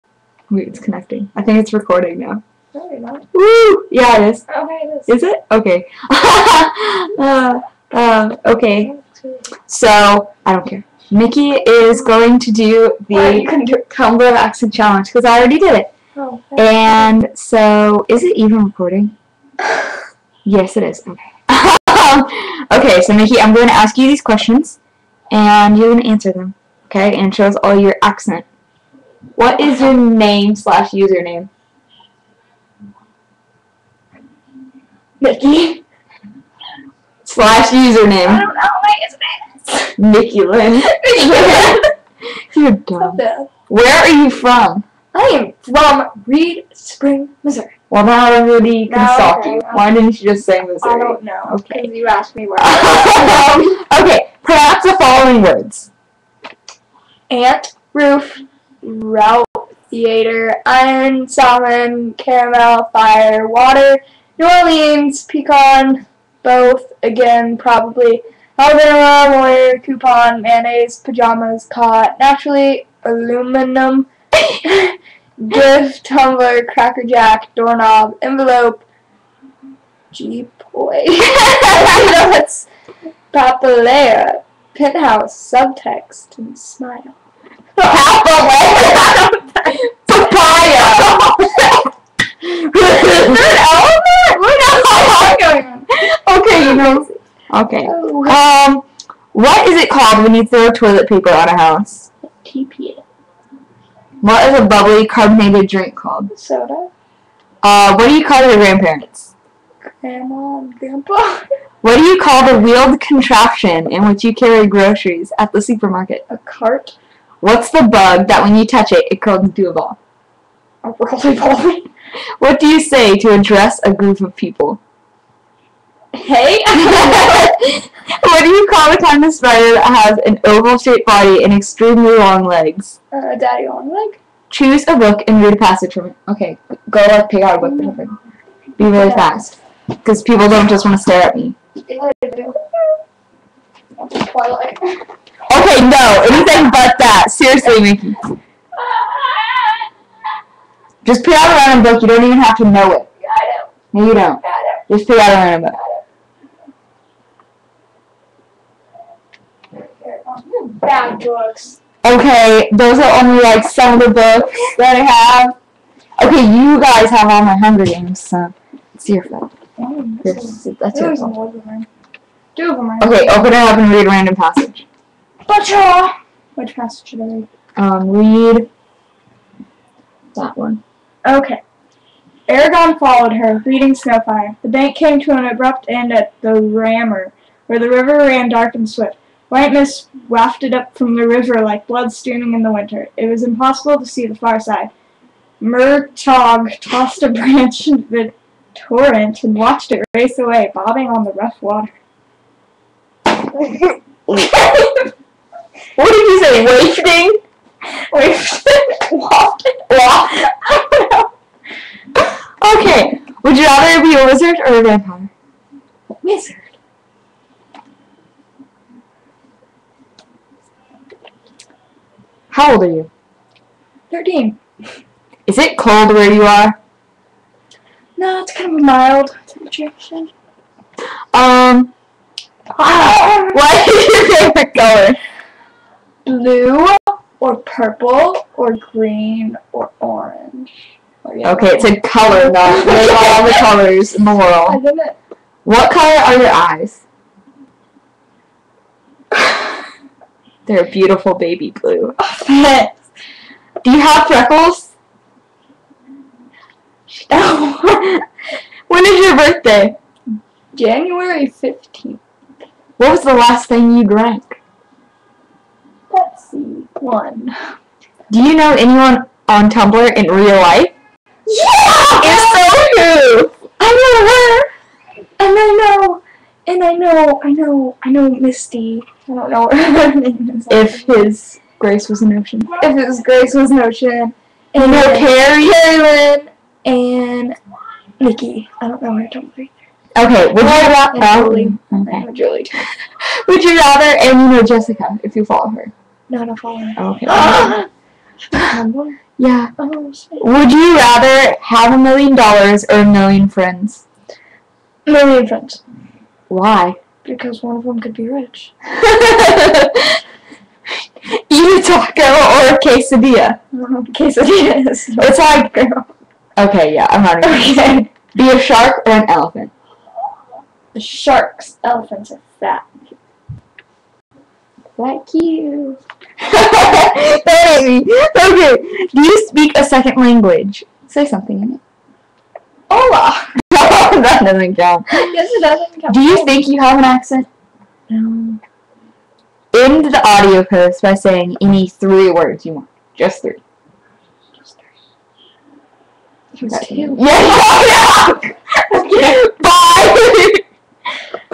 Mickey's Accent Challenge!
mickey does the accent challenge for tumblr :'D